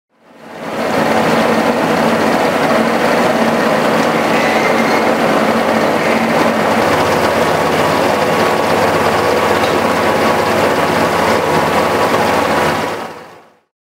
Fire Hose Maintenance machine
The maintenance begins with brushing the outside of the host.